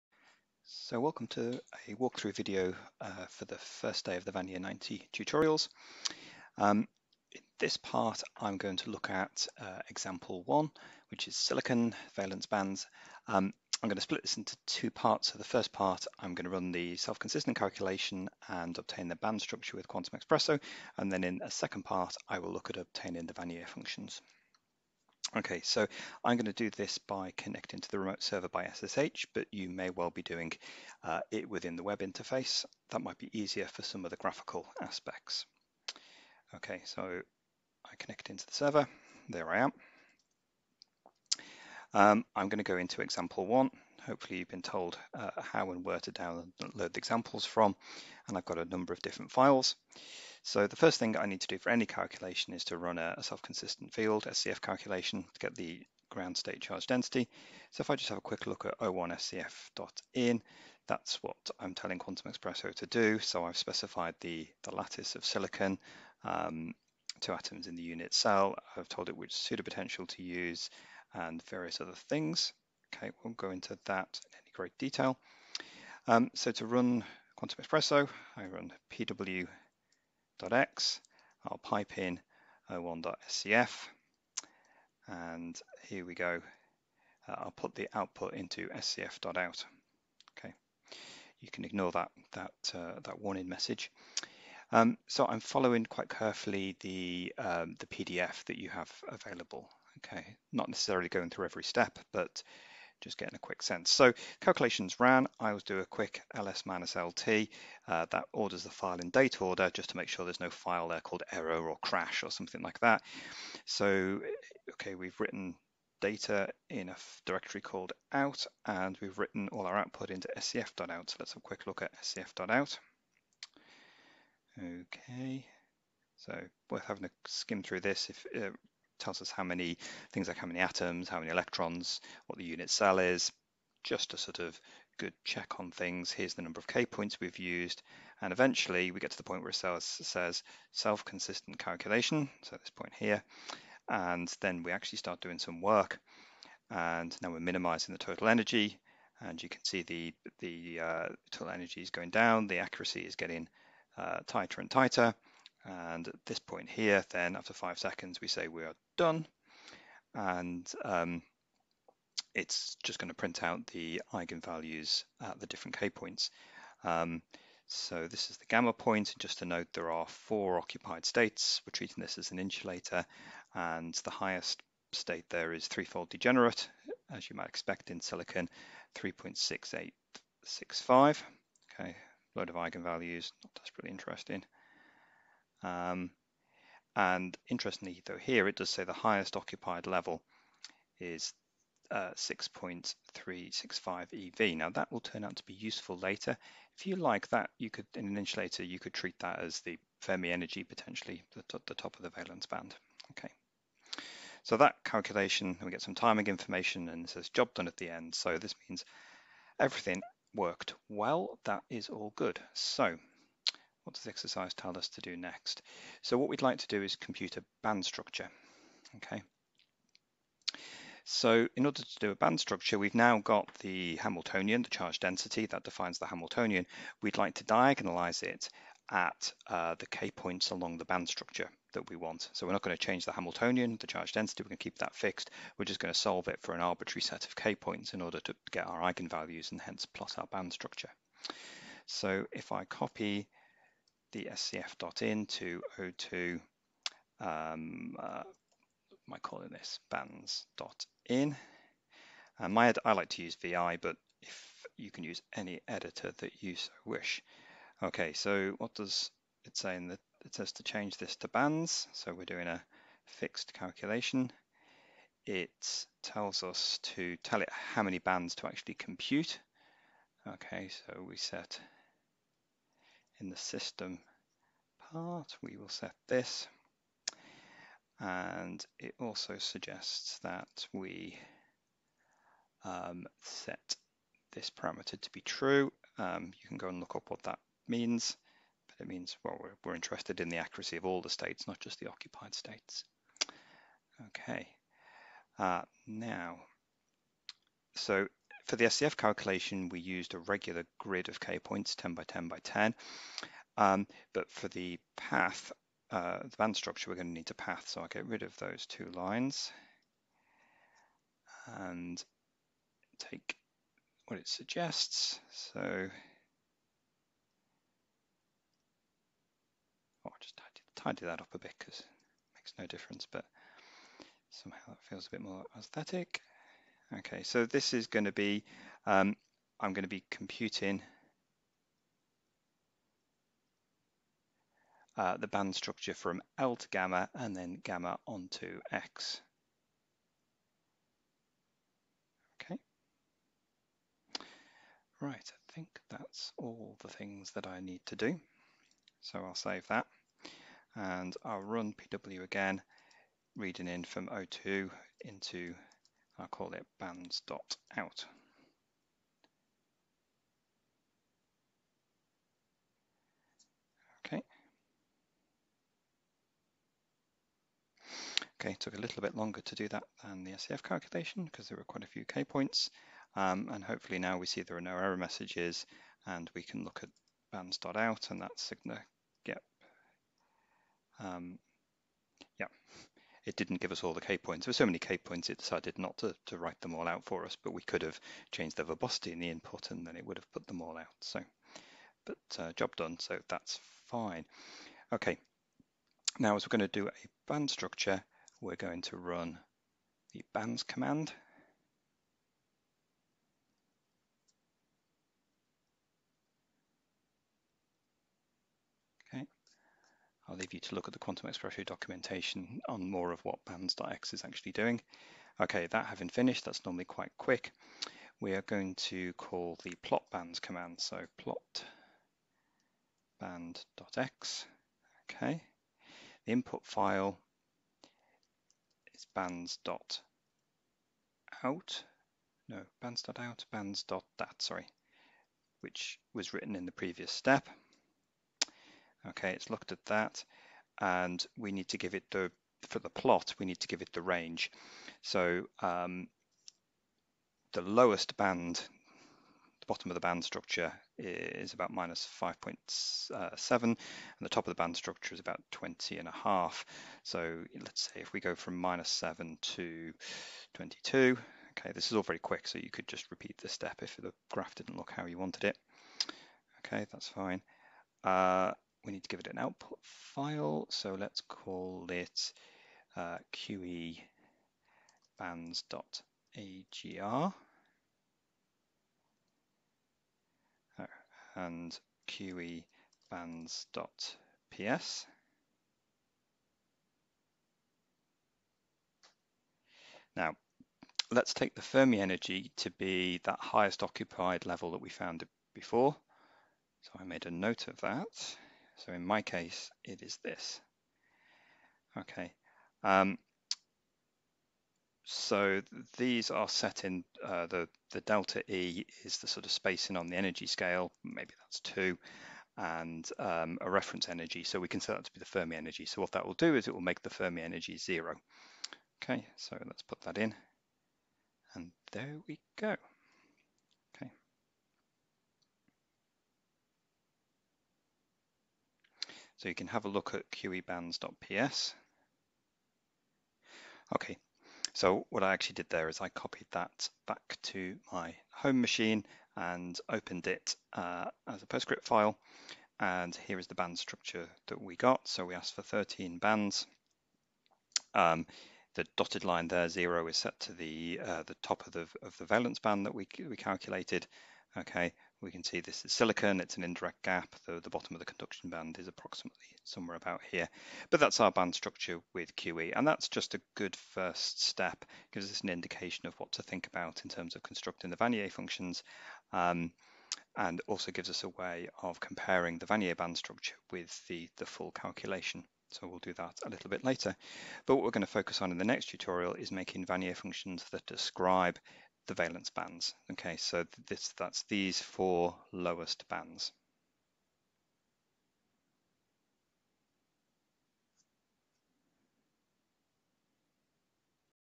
Wannier90 - Day1 - Ex1 - PtA
Walk through video for the 1st part of the Example 1 (Silicon).
In this part I will do the SCF calculation and plot the band structure

(Jonathan Yates, University of Oxford)